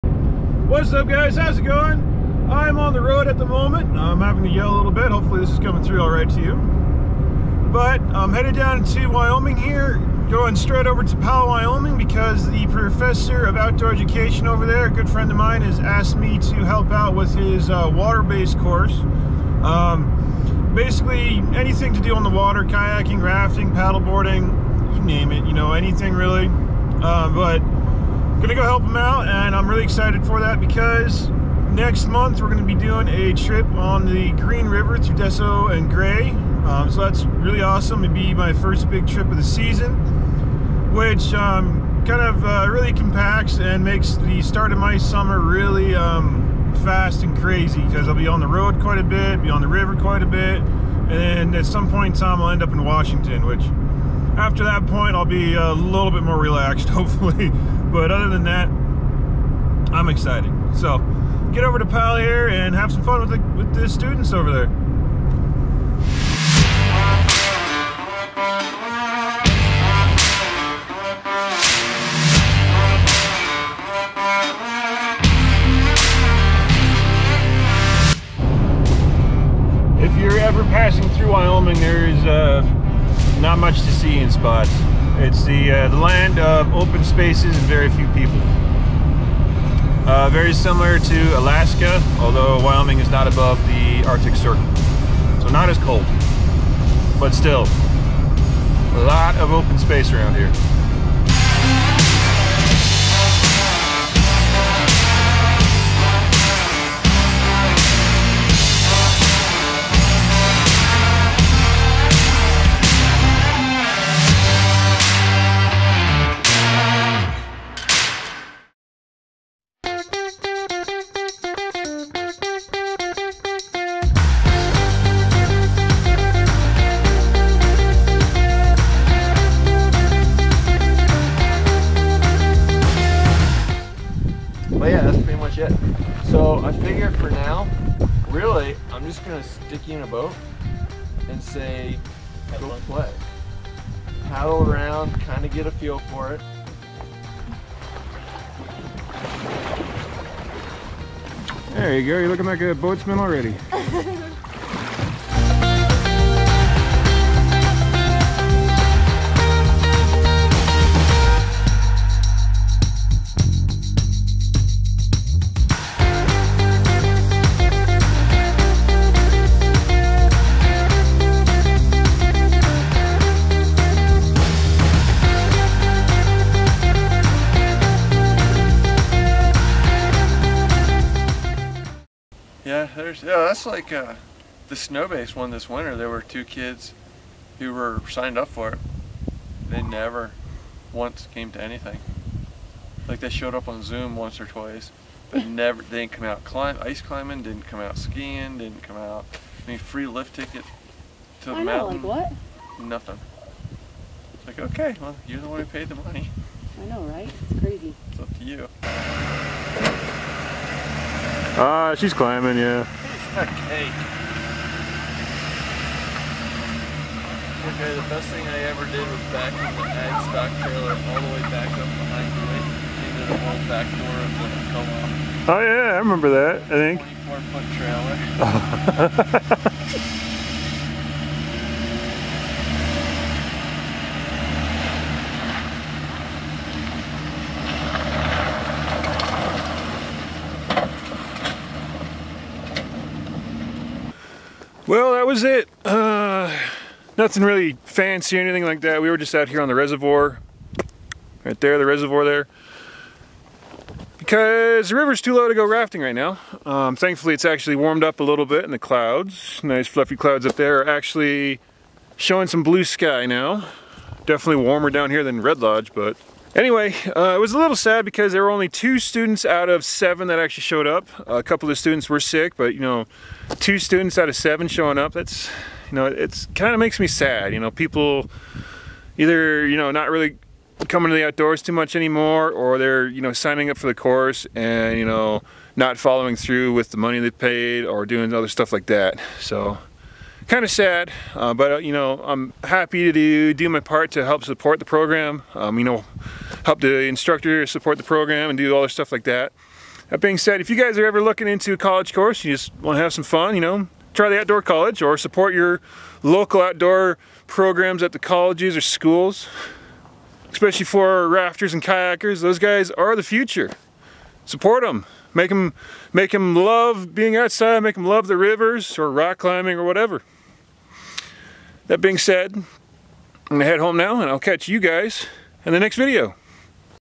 Paddling with my Old College
My old college asked me to help with their Outdoor Education Program. So, I headed back to College.